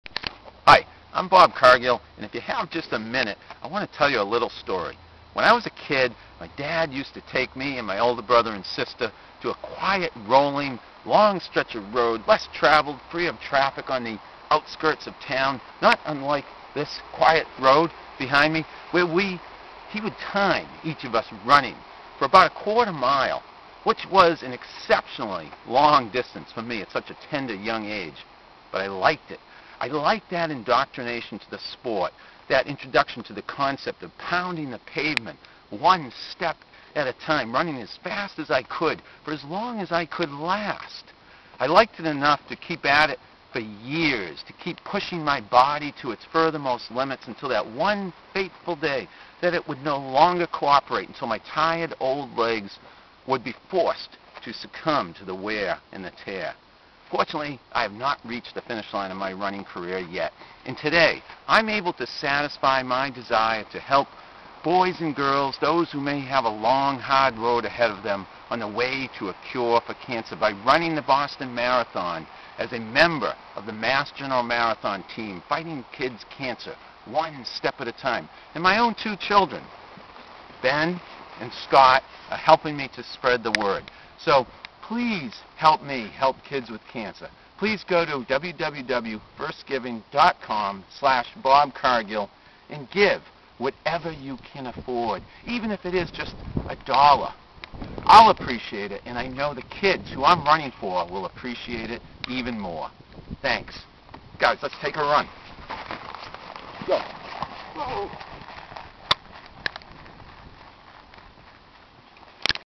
Bob's Running the Boston Marathon to Help Fight Kids' Cancer IV
When I was a kid, my dad used to take me and my older brother and sister to a quiet, rolling, long stretch of road  less travelled, free of traffic, on the outskirts of town  where he would time each of us running for about a quarter mile, which was an exceptionally long distance for me at such a tender, young age.

But I liked it. I liked that indoctrination to the sport, that introduction to the concept of pounding the pavement one step at a time, running as fast as I could go for as long as I could last. I liked it enough to keep at it for years, to keep pushing my body to its furthermost limits until that one fateful day that it would no longer cooperate, until my tired, old legs would be forced to succumb to the wear and the tear.

Fortunately, I have not reached the finish line of my running career yet. That day has not arrived. In fact, having recently entered my sixth decade of life, I'm happy to say that I'm still running 5Ks, 10Ks, marathons and mountains. I'm still running for time.

Yes, ever since a father taught his children how to run like the wind, one of them has run some 15,000 miles and is still going strong. And today, I'm able to satisfy my desire to help boys and girls  those who may have a long, hard road ahead of them on their way to a cure for cancer  by running the Boston Marathon as a member of the MassGeneral Marathon Team Fighting Kids' Cancer...One Step at a Time.

Please help me help kids with cancer.  and give whatever you can afford, even if it is just a dollar.  Ill appreciate it.  But the kids Im running for will appreciate it even more.  Thanks.